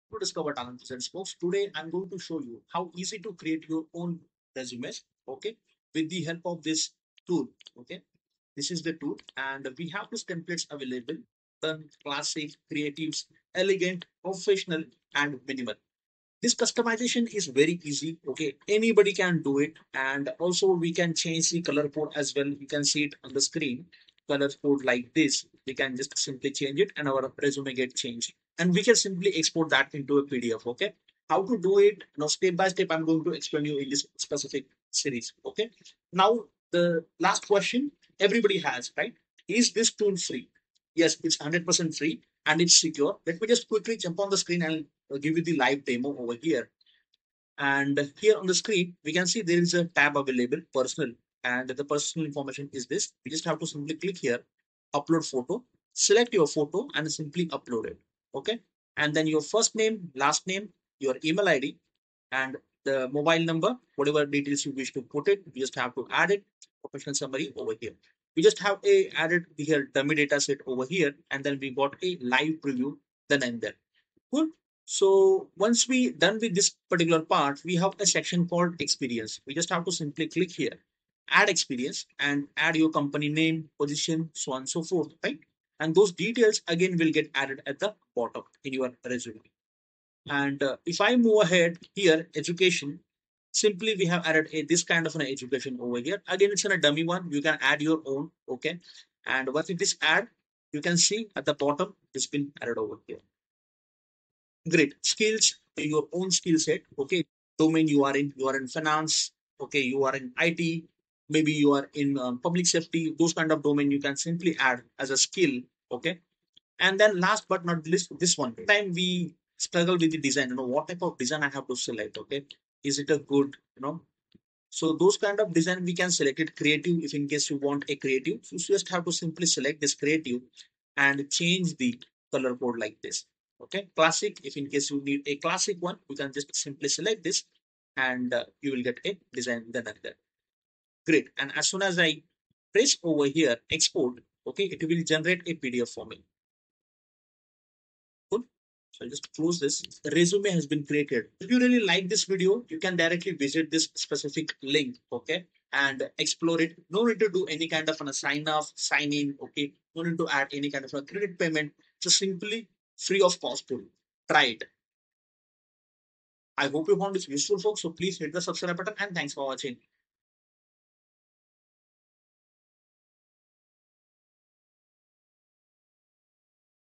Resume in 60s
Resume in 60s  Create Resume Free Learn how to easily create your own professional resume using a free and secure tool. This video shows you how to choose from various templates, including Classic, Creative, Elegant, Professional, and Minimal. You can also change the color scheme and export your resume as a PDF. The tool does not require you to sign up or provide any payment information.
00:00:00-00:00:06: Introduction to creating a resume
00:00:06-00:00:15: Overview of available templates and customization options
00:00:16-00:00:23: Confirmation that the tool is 100% free and secure
00:00:48-00:01:13: How to add personal information and a professional summary
00:01:17-00:01:30: Adding your work experience
00:01:30-00:01:43: Adding your educational background
00:01:44-00:01:56: How to include your skills
00:02:22-00:02:32: Exporting the final resume as a PDF